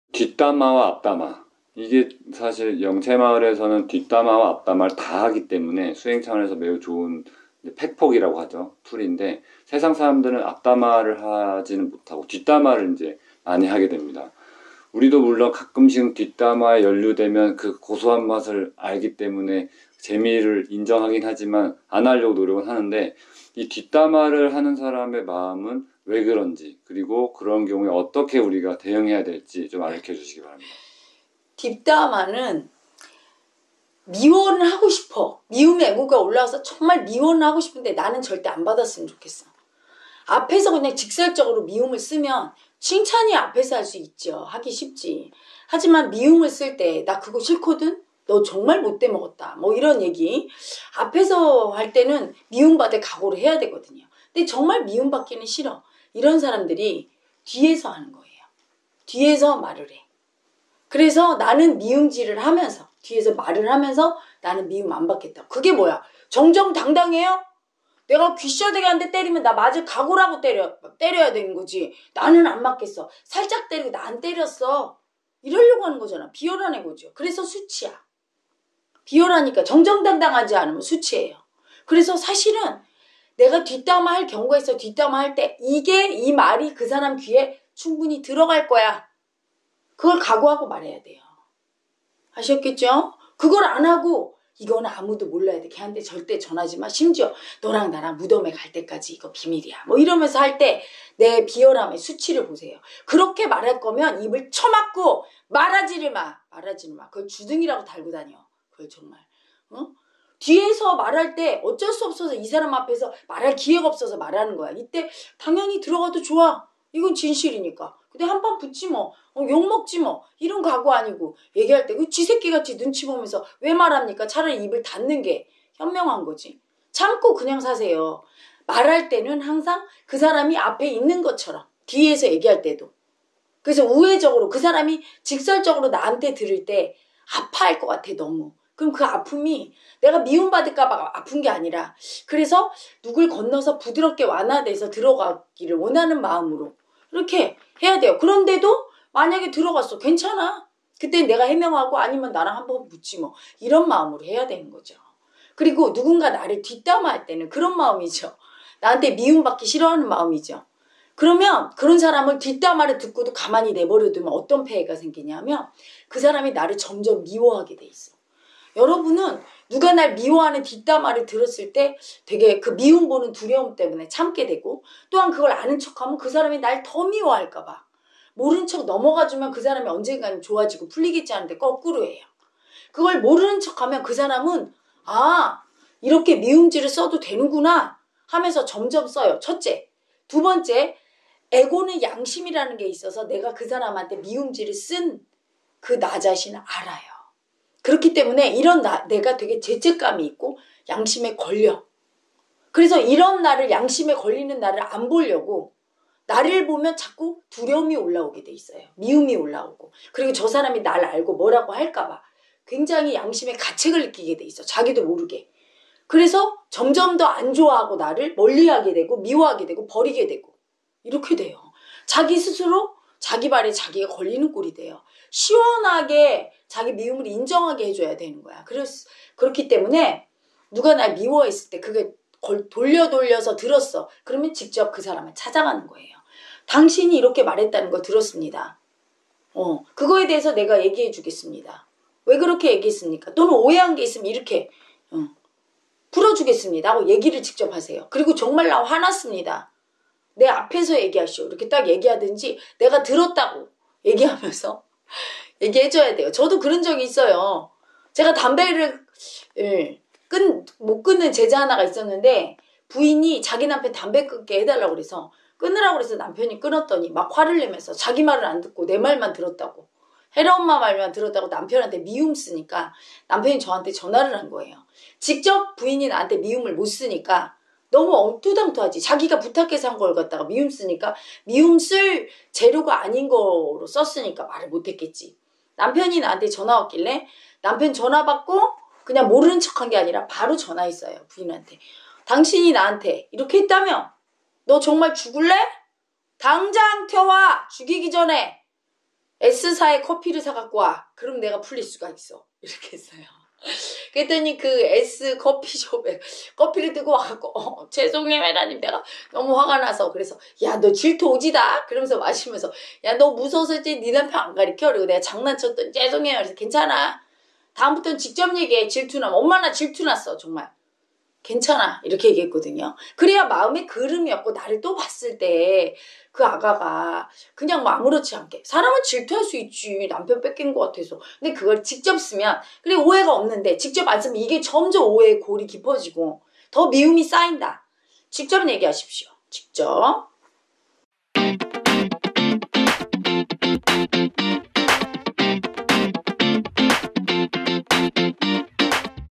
뒷담화 하는 사람의 심리와 대응법
뒷담화에 종종 빠지는 우리!
불가피하게 뒷담화를 하게 된다면 이런 마음으로 해라.
그리고 뒷담화를 하는 사람에게는 이렇게 대응하라.
삶의 지혜를 혜라엄마가 알려줍니다.